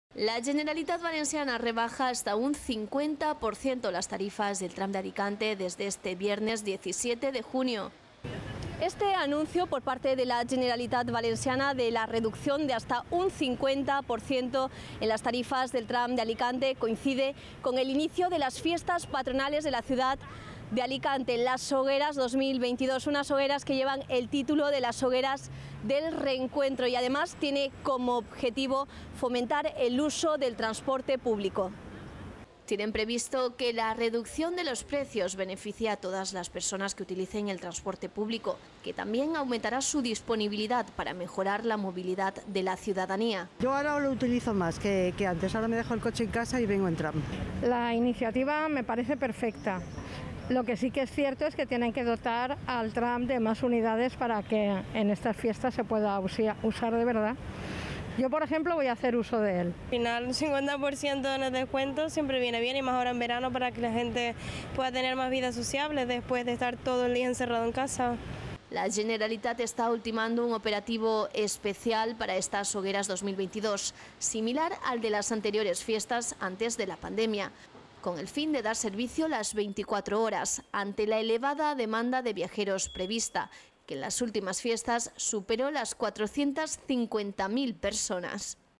A partir del 17 de junio el Tram de Alicante tendrá una reducción de hasta el 50%
La Generalitat Valenciana rebaja hasta un 50 % las tarifas del TRAM de Alicante desde el viernes 17 de junio.

Tienen previsto que la reducción de los precios beneficie a todas las personas que utilicen el transporte público, que también aumentará su disponibilidad para mejorar la movilidad de la ciudadanía.

La Generalitat está ultimando un operativo especial para las Hogueras de 2022, similar al de las anteriores fiestas antes de la pandemia, con el fin de dar servicio las 24 horas, ante la elevada demanda de viajeros prevista, que en las últimas fiestas su cifra superó las 450.000 personas.